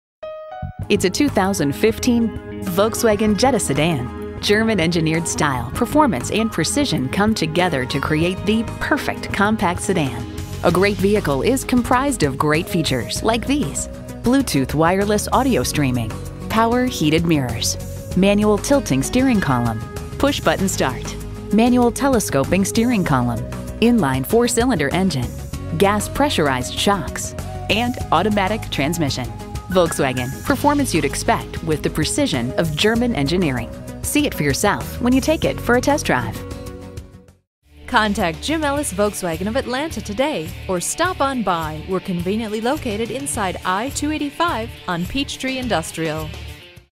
Used 2015 Volkswagen Jetta Atlanta Alpharetta, GA V3175 - SOLD
Year: 2015
Make: Volkswagen
Model: Jetta
Trim: 2.0L S
Engine: 2.0L I4 SOHC
Transmission: 6-Speed Automatic with Tiptronic
Color: Gray
Interior: Titan Black
Mileage: 28622
Stock #: V3175
VIN: 3VW2K7AJ5FM279709
Clean CARFAX. Certified. Jetta 2.0L S w/TechnologybrbrCLEAN CAR FAX, TOUCH SCREEN RADIO, LOCAL TRADE, BLUETOOTH, Jetta 2.0L S w/Technology, 6-Speed Automatic with Tiptronic, Platinum Gray Metallic, Titan Black w/Cloth Seat Trim, Radio: Premium VIII Touchscreen w/6-CD, Rear-View Camera. 34/23 Highway/City MPG

Address:
5901 Peachtree Industry Boulevard